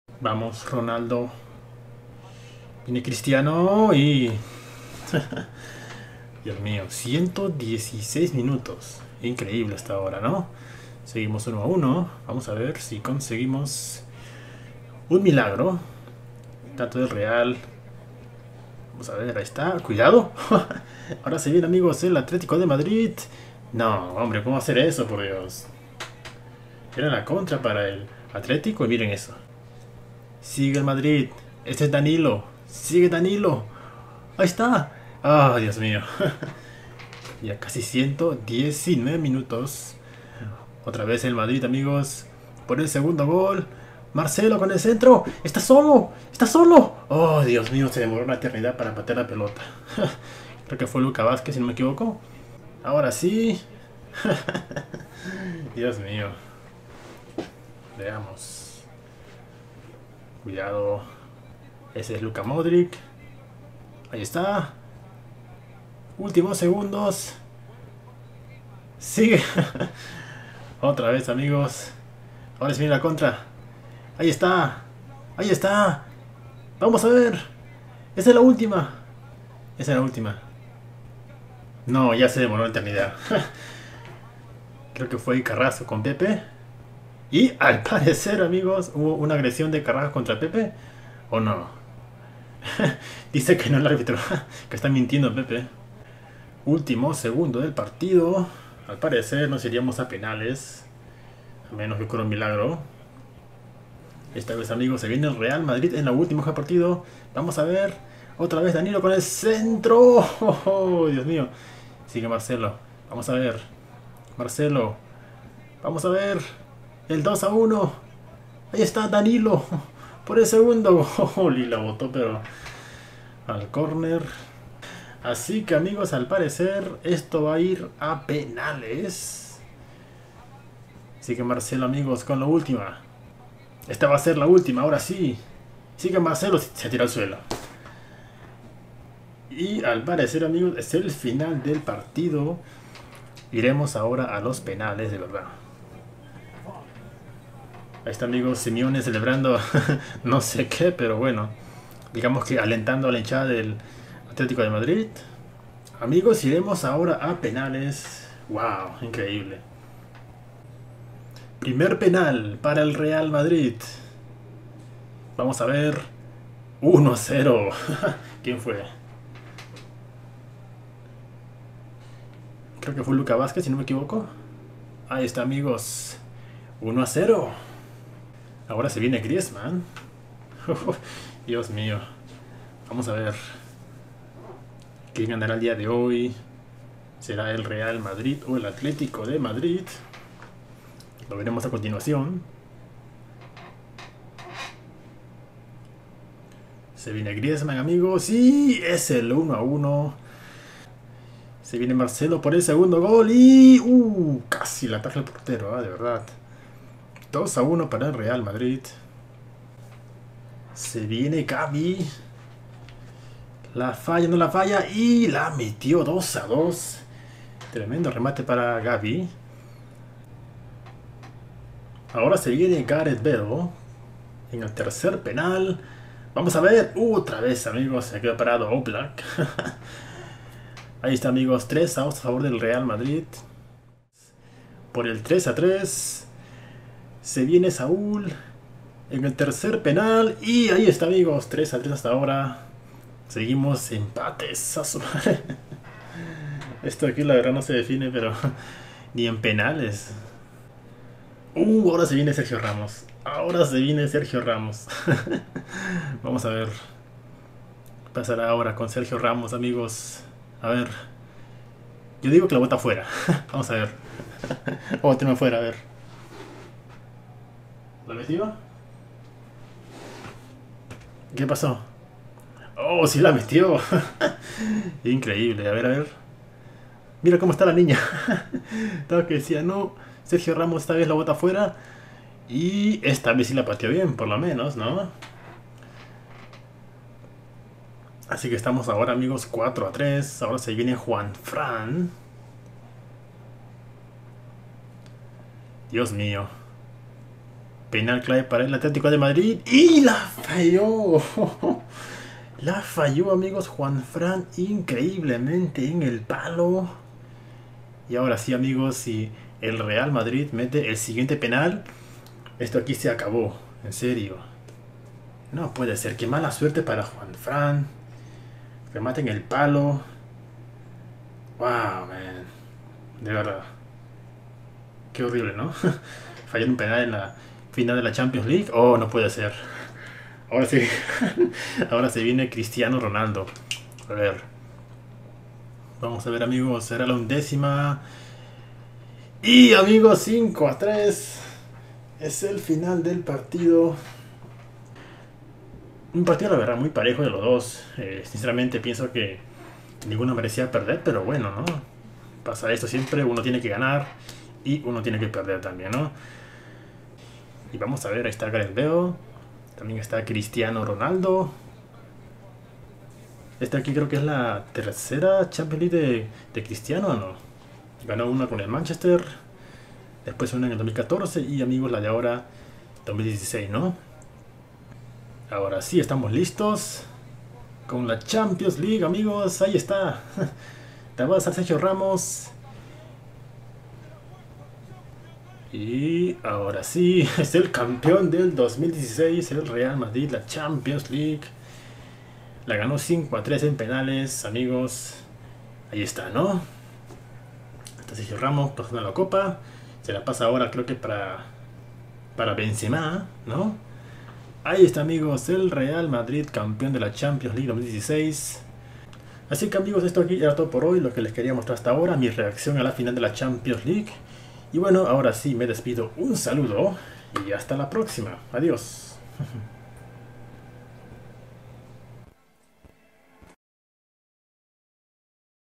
REACCION A REAL MADRID vs ATLETICO DE MADRID 1-1 (5-3) FINAL UEFA Champions League
Video Reaccion de la Final de la Champions League entre el Real Madrid y Atletico de Madrid

No te olvides de apoyar este video con un LIKE/Me Gusta, un saludo :)